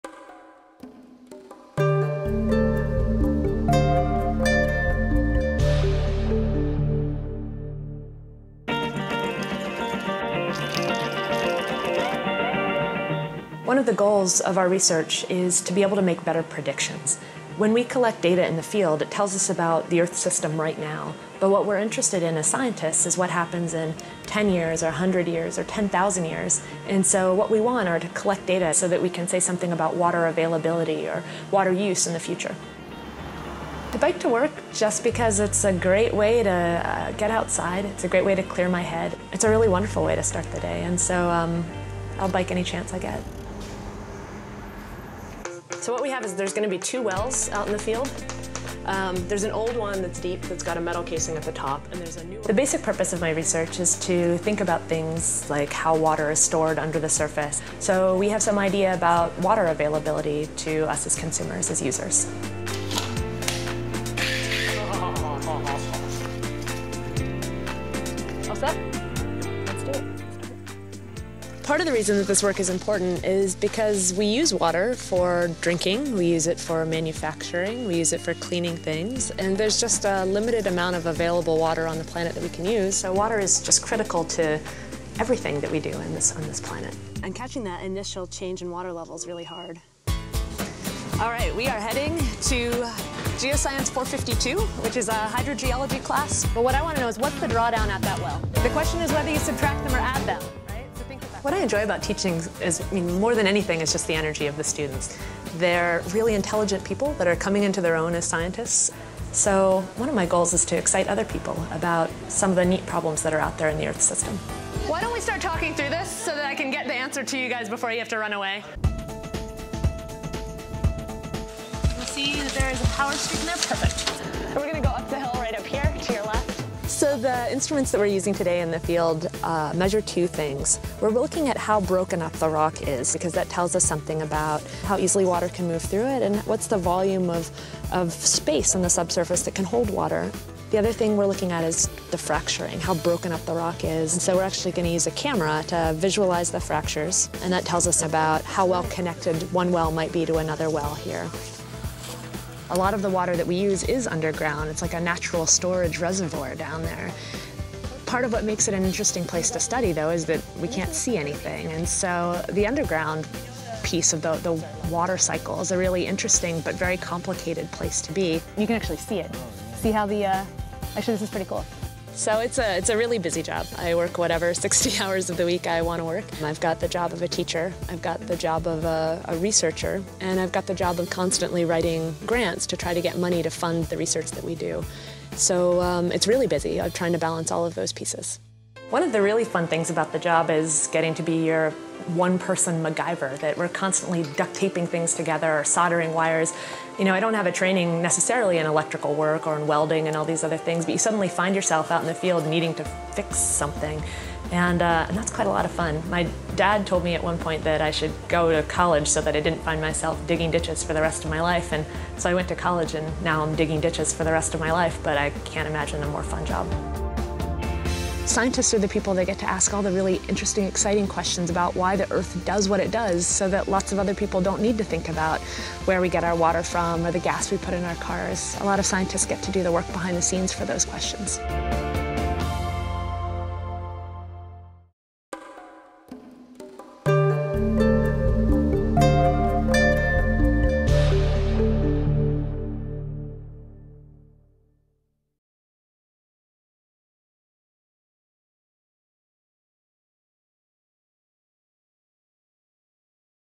Penn State - Kamini Singha - Real Scientist
This is the first of the Penn State RealScientist videos, showcasing Kamini Singha of the GeoSciences Department.